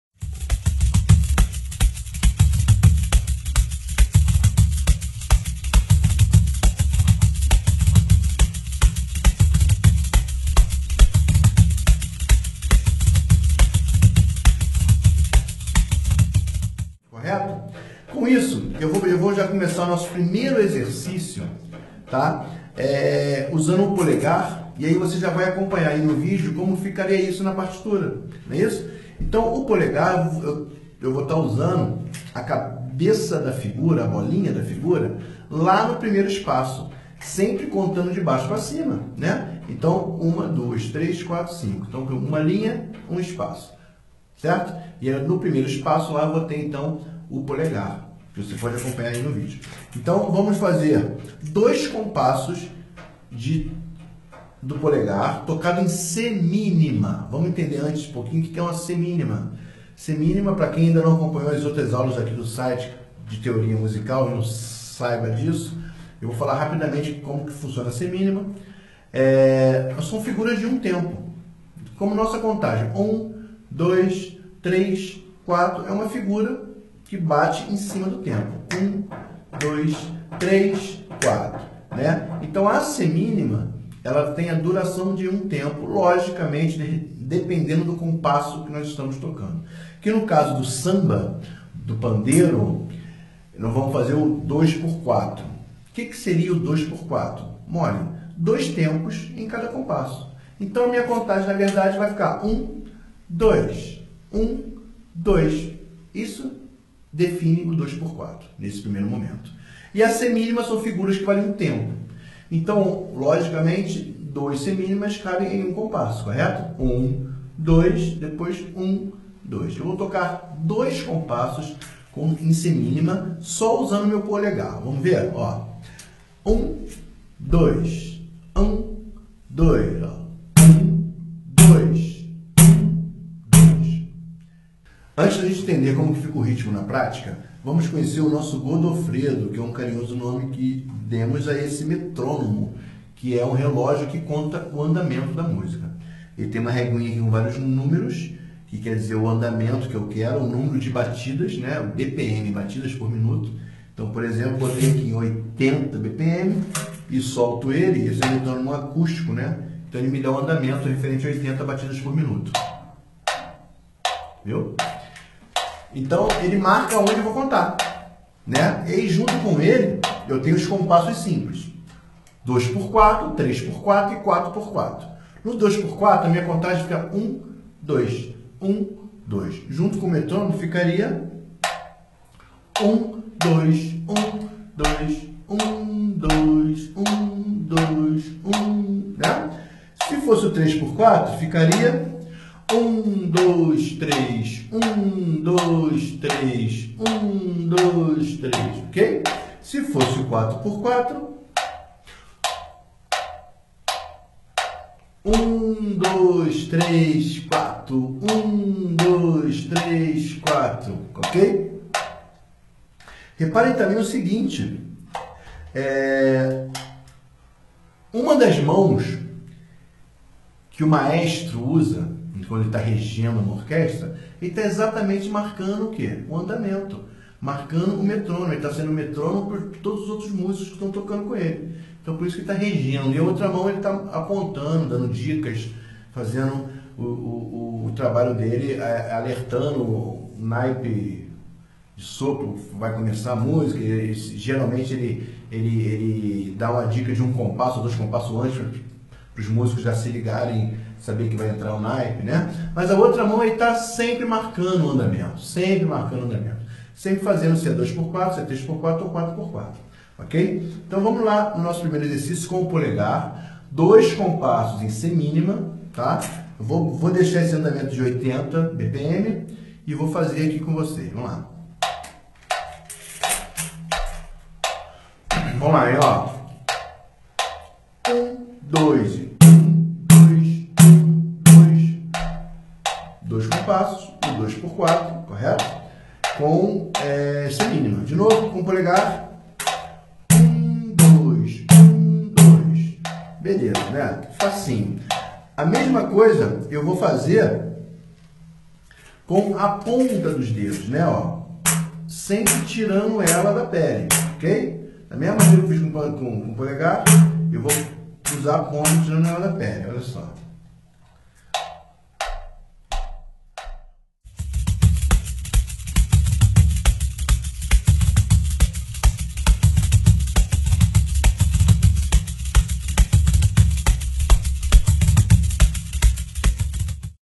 Percussão - Conhecendo o Pandeiro Parte 2 - Cordas e Música
AULA RESTRITA PARA ASSINANTES! Conheça na partitura e na prática os primeiros toques no pandeiro! Aula com o Prof. Anderson Sabadine.
Imprima os seus exercícios em nosso site oficial!

Entre e conheça o site oficial de nossa Escola de Música Virtual Cordas e Música.